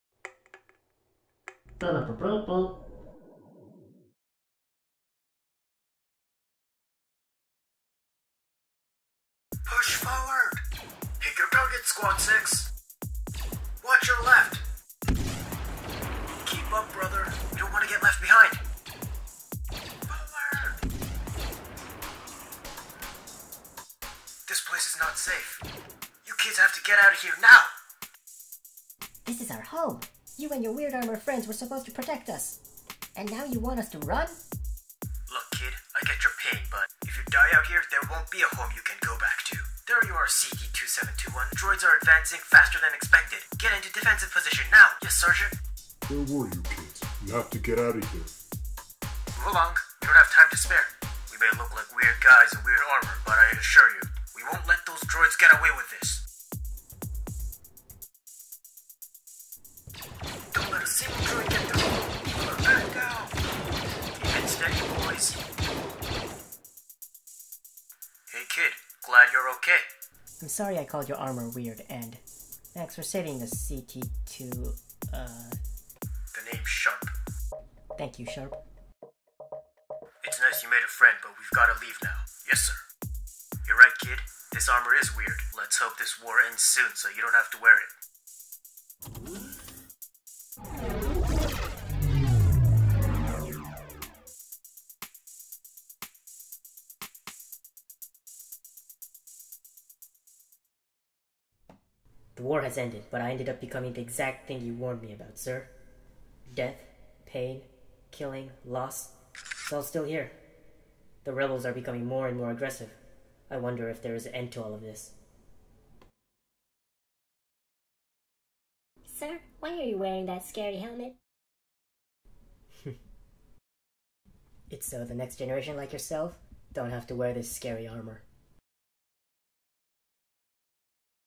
-Baton of Hope- Lego Star Wars Stop-Motion
From the perspective of the everyday citizen of the Star Wars universe, battle droids are merciless killing machines and clone troopers are faceless cold solders. History may repeat itself but for each change, each person has a choice to be better and have hope for the future.

This is a project I filmed and edited in 3 days to enter Gold Puffin's 10K Stop Motion Contest.